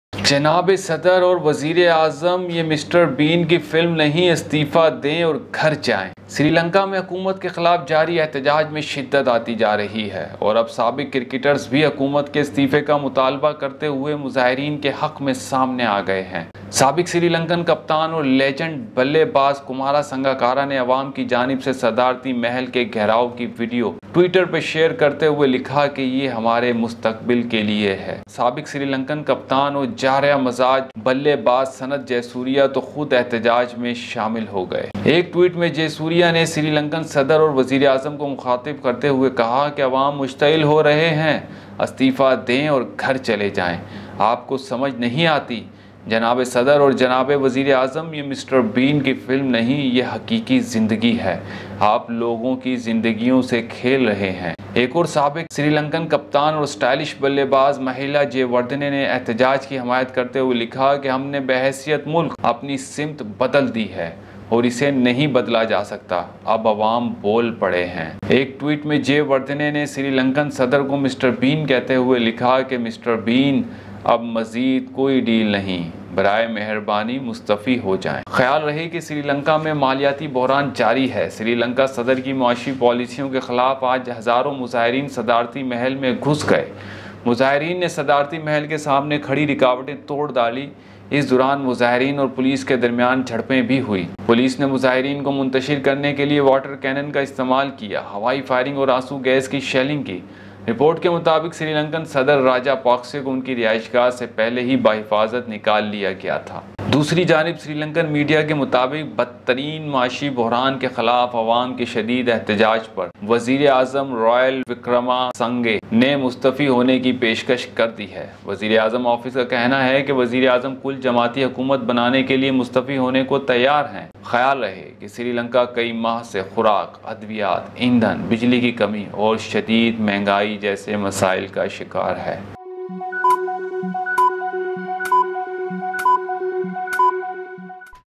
SRI LANKAN CRICKETERS | 'جناب صدر اور وزیراعظم! یہ مسٹر بین کی فلم نہیں، استعفیٰ دیں اور گھرجائیں'
SRI LANKA CRISIS | CRICKETERS ON ROAD | سری لنکا میں حکومت کے خلاف جاری احتجاج میں شدت آتی جارہی ہے اور اب  سابق کرکٹرز  بھی حکومت کے استعفےکا مطالبہ کرتے ہوئے مظاہرین کے حق میں سامنے آگئے ہیں۔